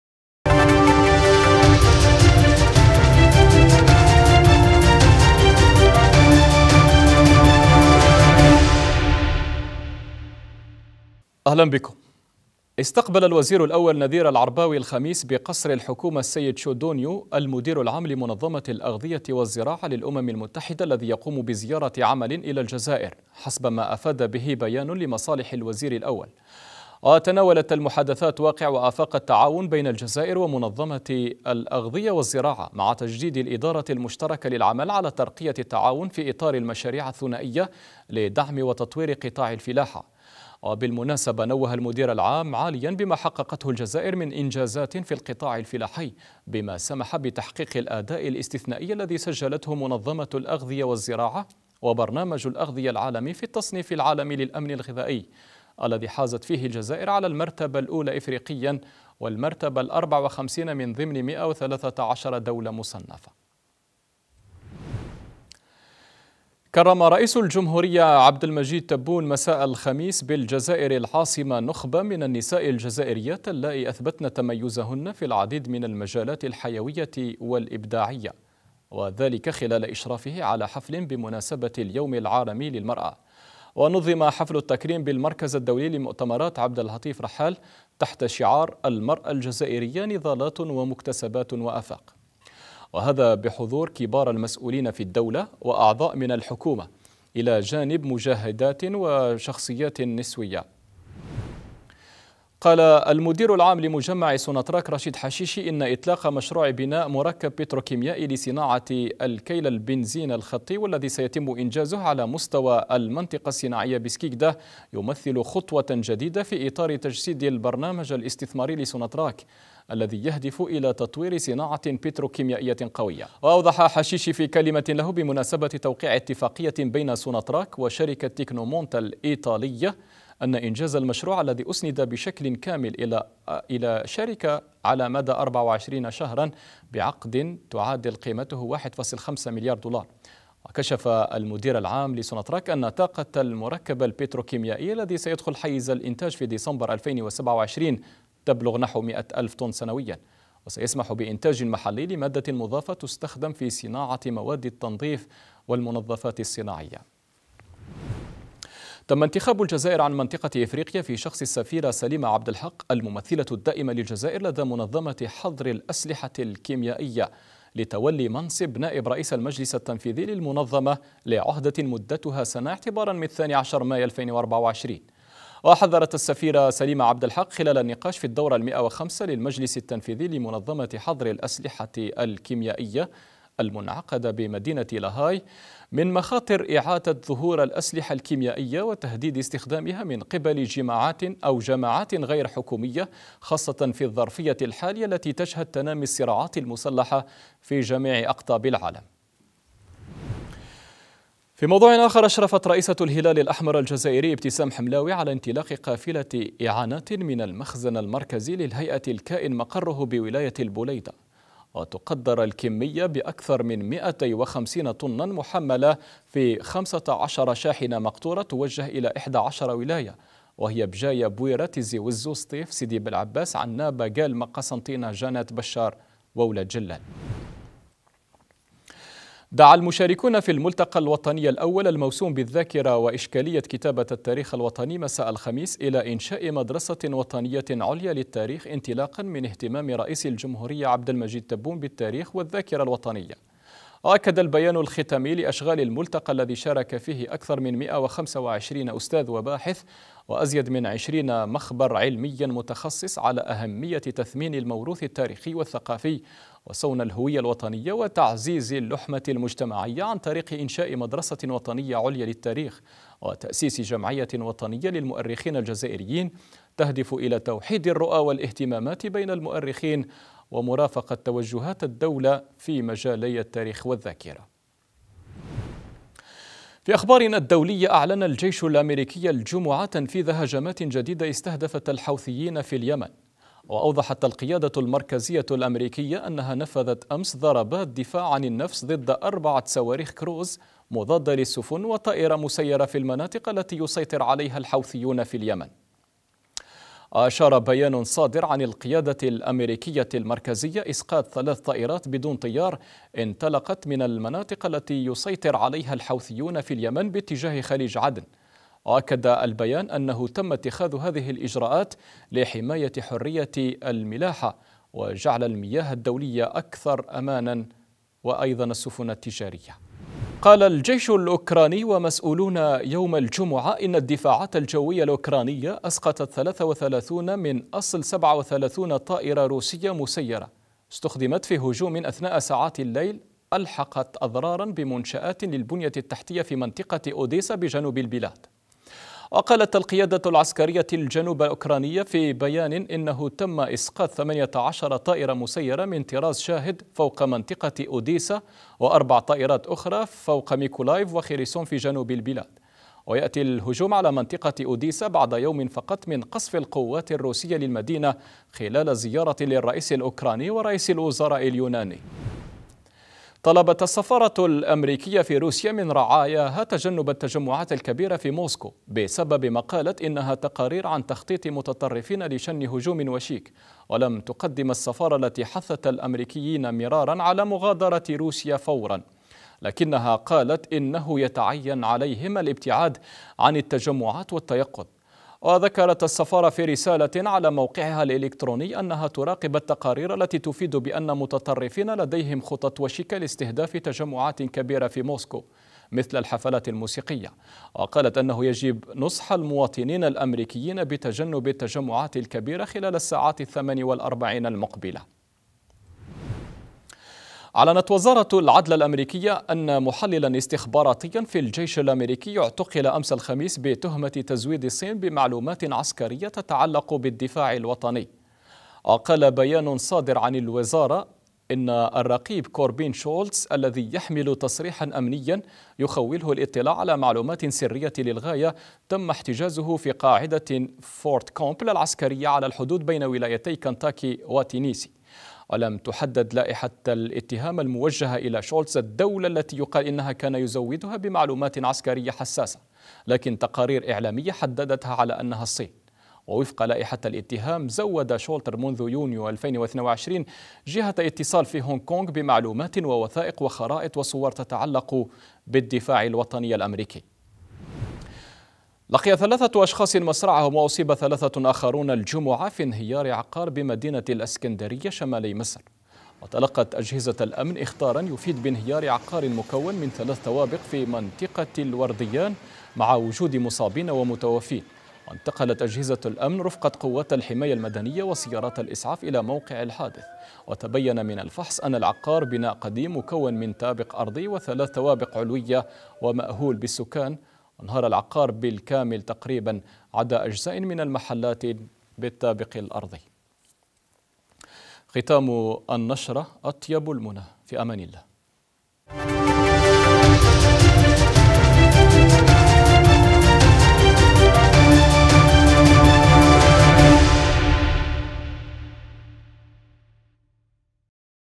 تكريم الرئيس تبون لنخبة من النساء الجزائريات بمناسبة عيد المرأة أبرز مافي نشرة أخبار اليوم
تكريم الرئيس تبون لنخبة من النساء الجزائريات بمناسبة عيد المرأة وكذا شراكة اقتصادية بين الجزائر وإيطاليا في مجال المحروقات  أبرز مافي نشرة أخبار اليوم